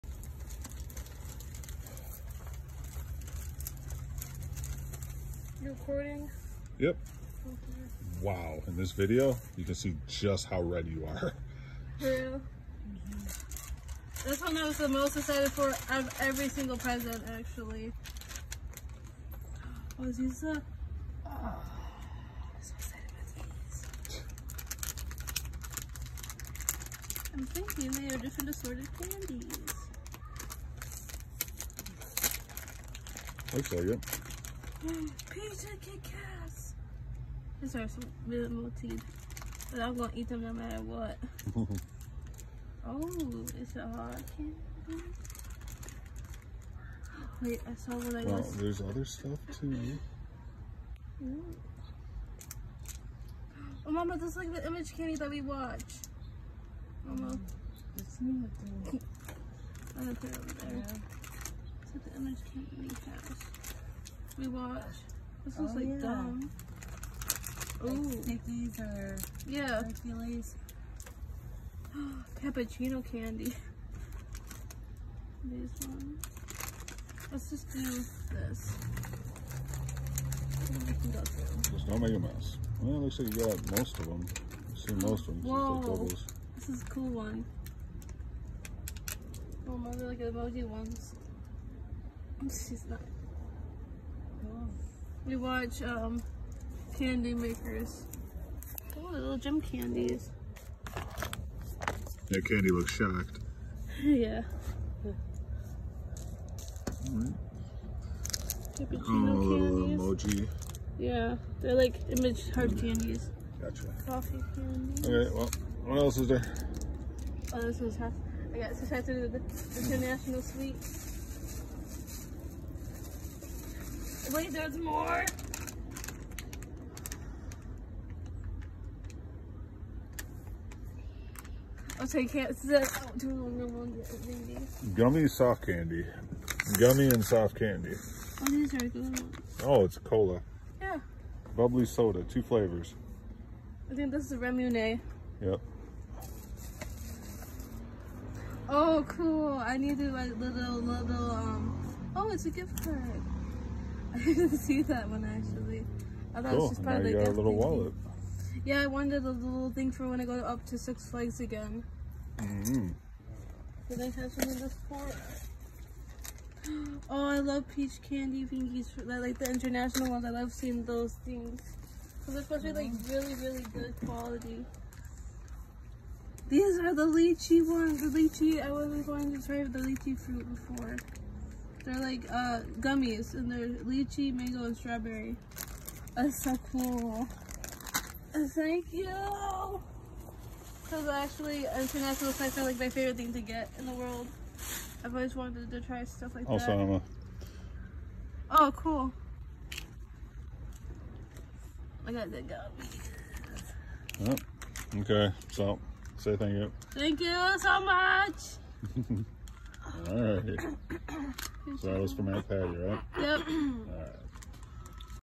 Opening gifts from Aunt Patty
Now we know why the trip to China town lol